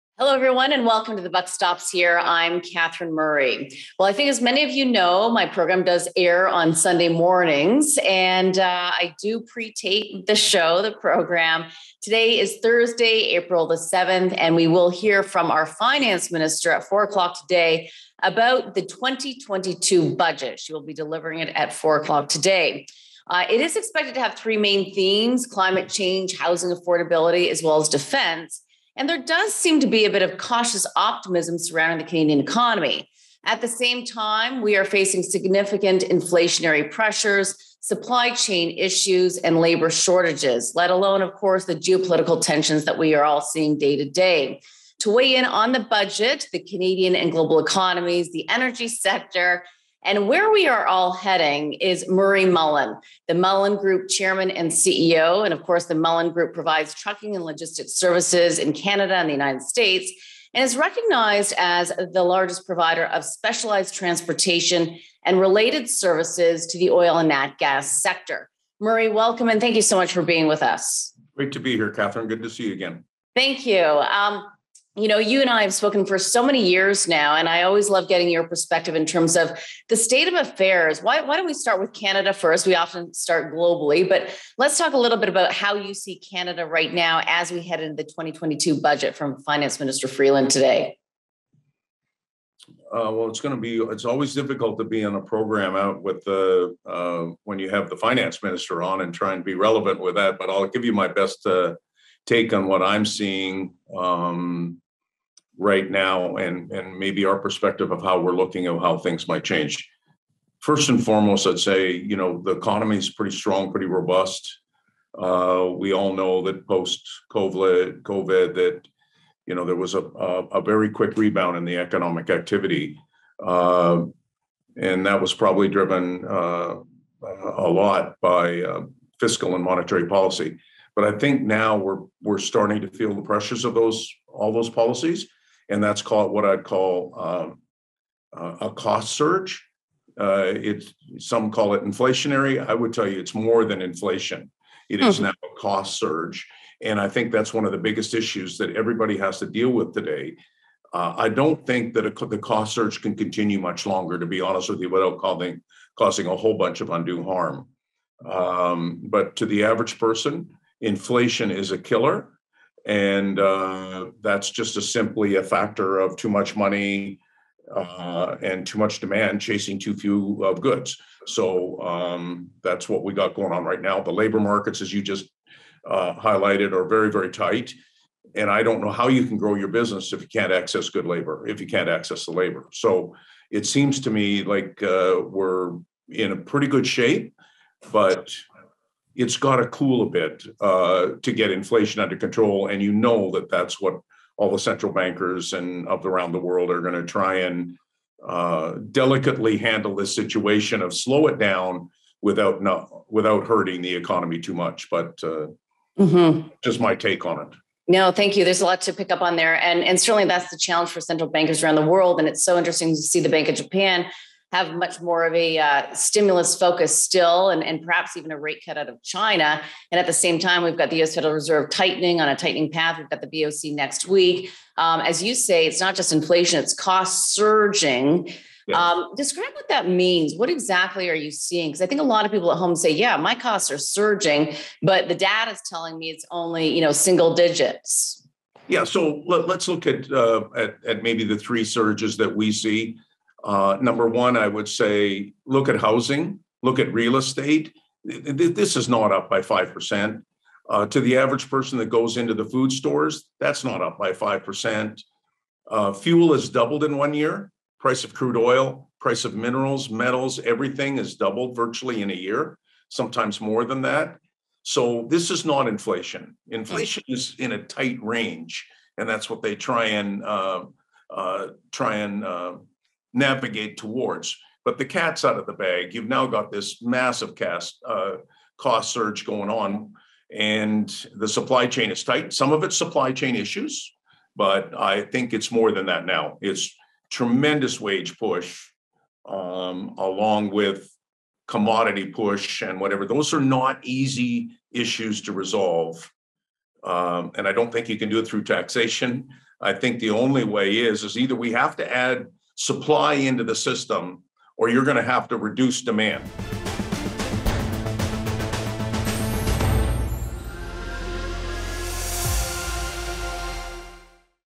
This Is More Than Just Inflation
Catherine welcomes Murray Mullen, CEO of the Mullen Group, to discuss the biggest issues that need to be addressed in the next Federal Budget.